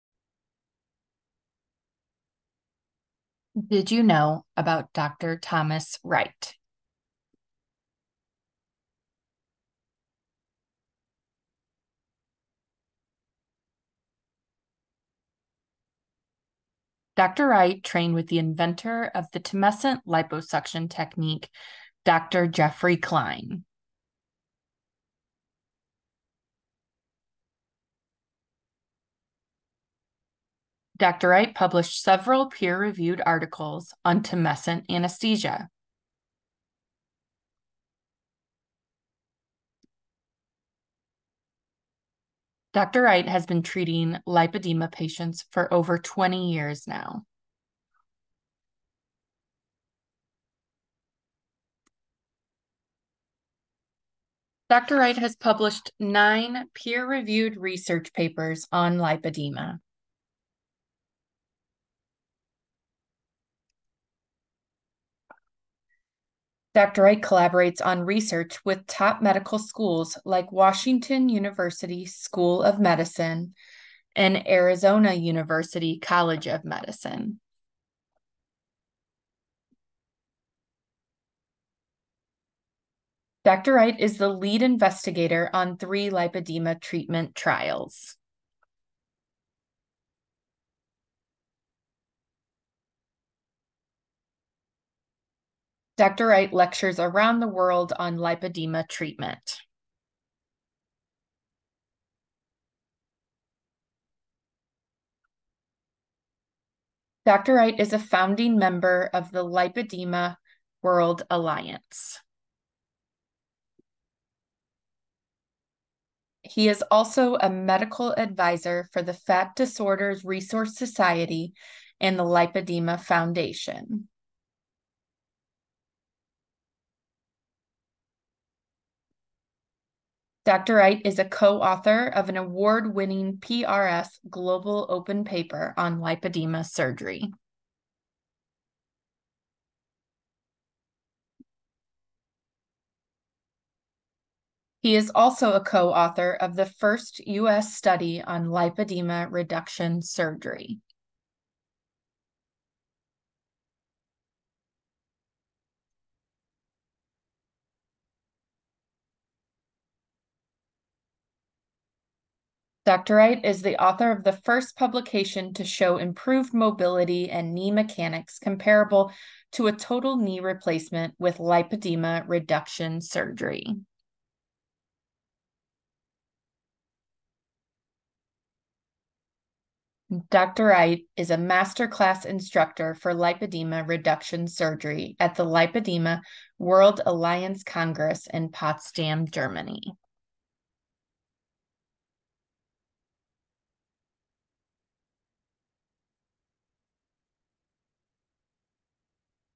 Dr. Thomas Wright: Leading Innovator in Lipedema Treatment and Tumescent Liposuction | Achievements
Join us as we delve into the remarkable achievements of Dr. Thomas Wright, a renowned expert in the field of lipedema treatment and a pioneer in tumescent liposuction. Dr. Wright's outstanding accomplishments include training with the inventor of tumescent liposuction, publishing numerous peer-reviewed articles on the subject, and dedicating over two decades to treating lipedema patients.

With an impressive record of nine peer-reviewed papers on lipedema and collaborations with top medical schools, Dr. Wright is at the forefront of innovative research and treatment. He also serves as the lead investigator in pivotal lipedema treatment trials, paving the way for advancements in patient care.

Furthermore, Dr. Wright's expertise has earned him invitations to lecture around the world, sharing his knowledge on lipedema. Notably, he authored the first publication to demonstrate significant improvements in mobility and knee mechanics comparable to total knee replacement through lipedema reduction.

Learn more about Dr. Thomas Wright, a leading figure in the field of lipedema treatment, and discover the groundbreaking work he has contributed to the medical community and watch more videos on our channel.